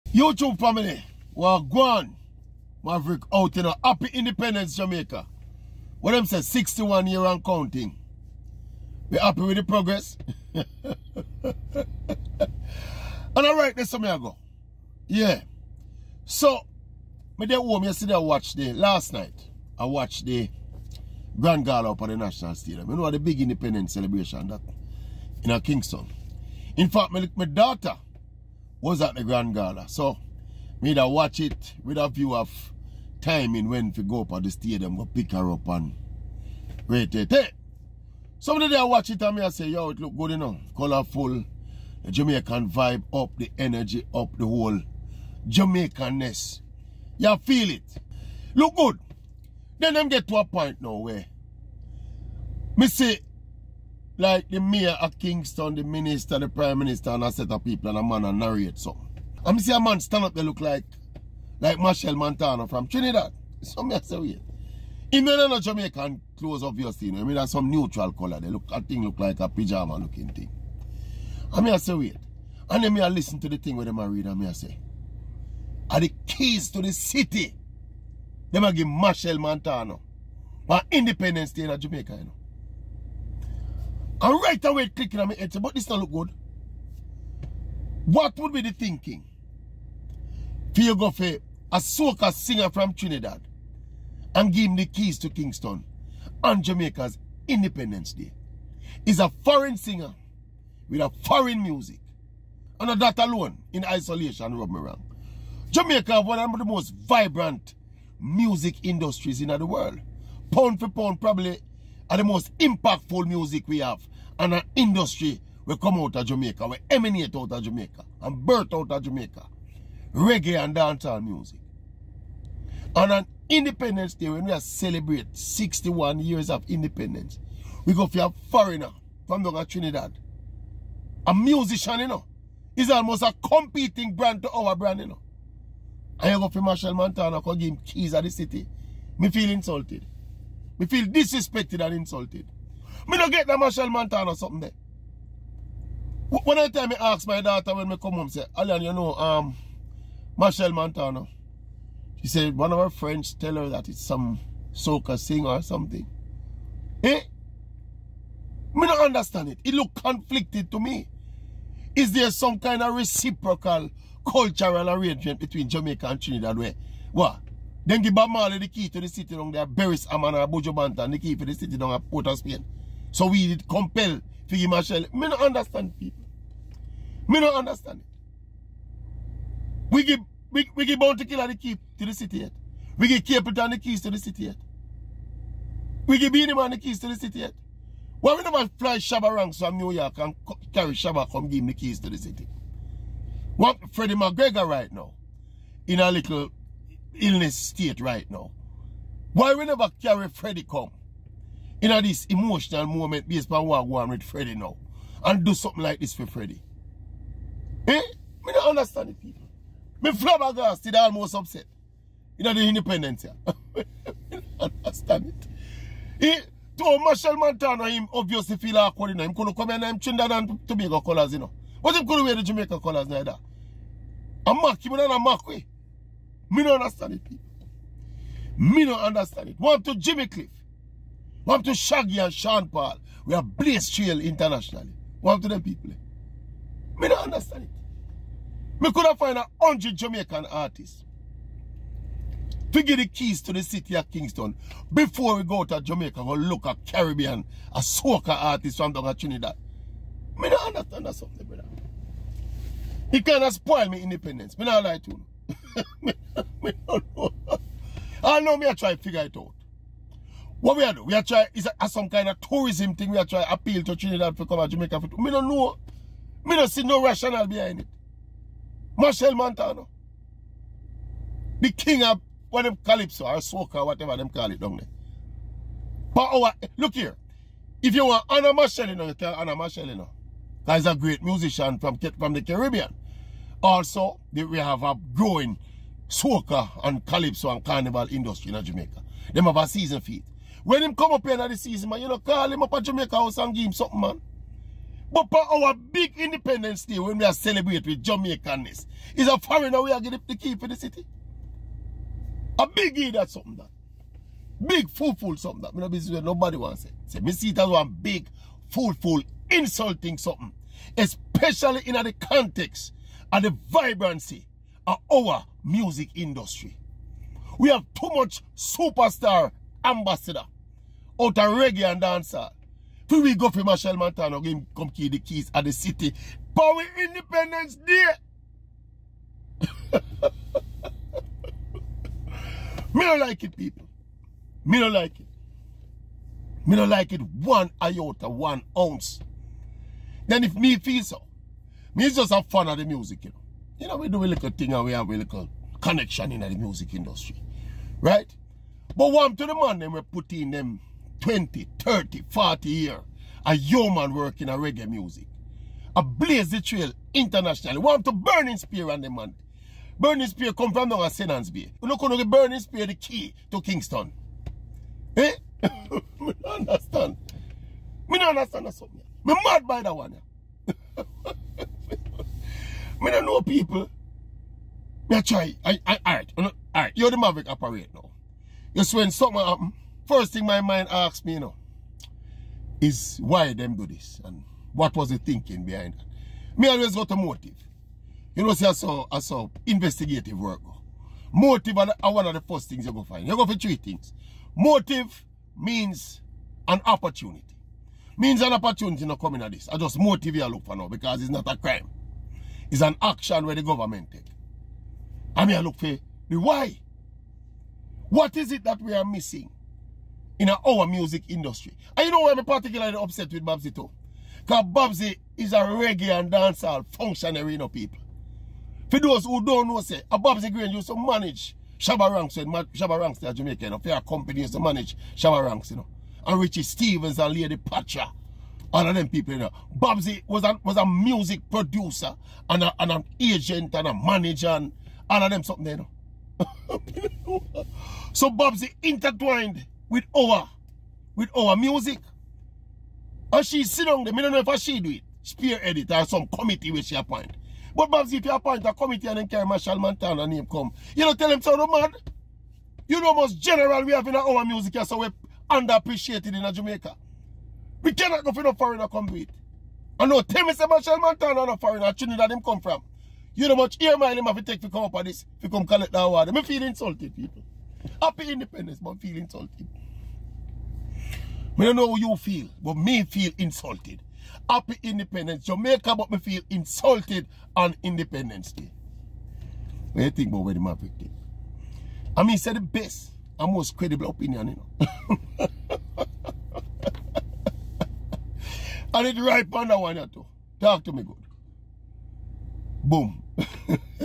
JAMAICA INSULTED AT GRAND GALA!!
PRESENTING THE KEYS TO THE CITY OF KINGSTON TO TRINIDADIAN MACHEL MONTANO ON JAMAICA'S INDEPENDENCE DAY WAS NOT A GOOD LOOK.